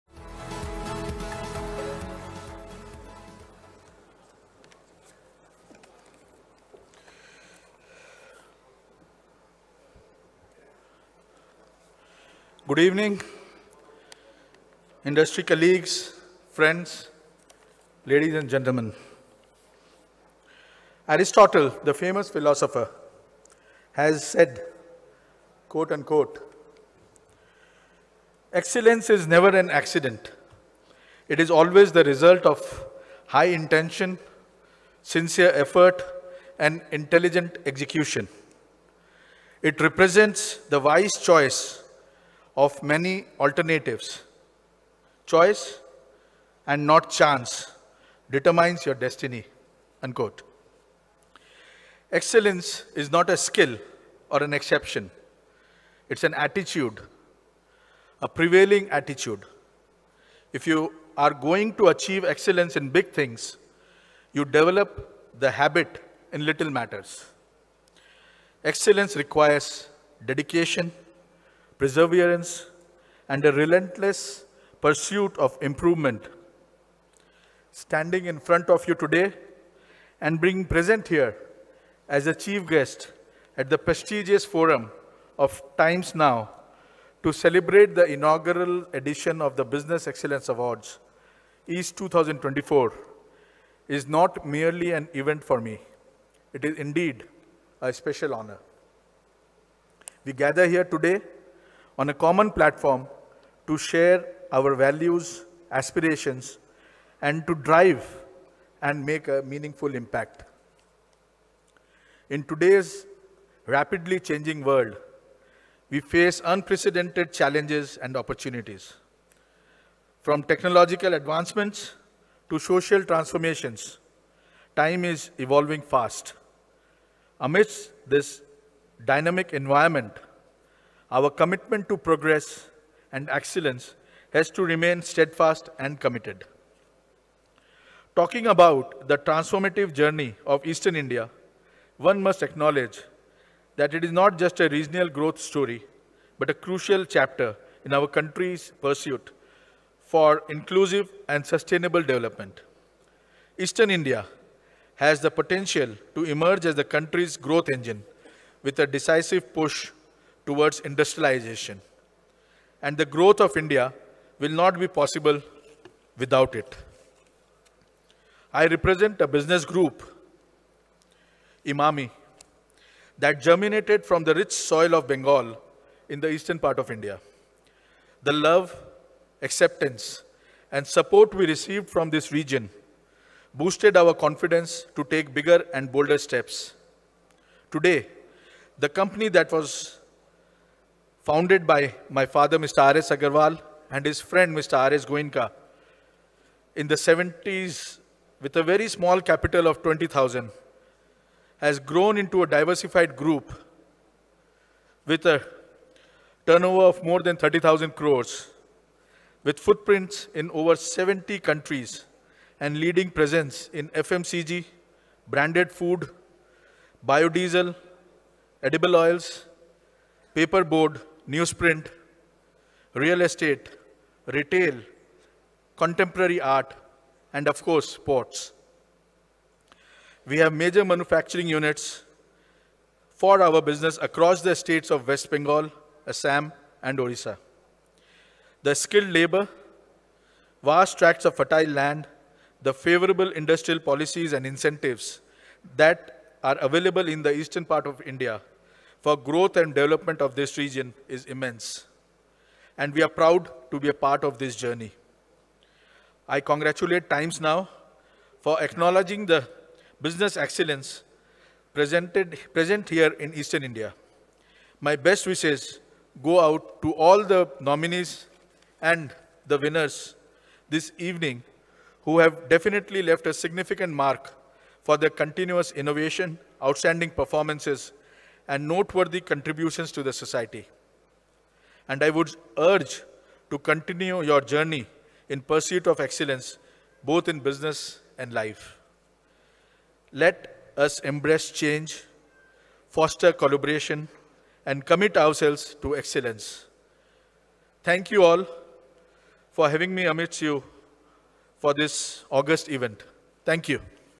Aditya V Agarwal, Promoter Director of Emami Group, Speaks At Business Excellence Awards 2024
Aditya V Agarwal, Promoter Director of Emami Group, spoke at Times Now Business Excellence Awards 2024. The award ceremony celebrated the achievements of the most innovative and influential young leaders in the business world. Attended by the key figures of biz world, 'TimesNow.in Business Excellence Awards - East 2024' took place on 19th June at ITC Royal Kolkata.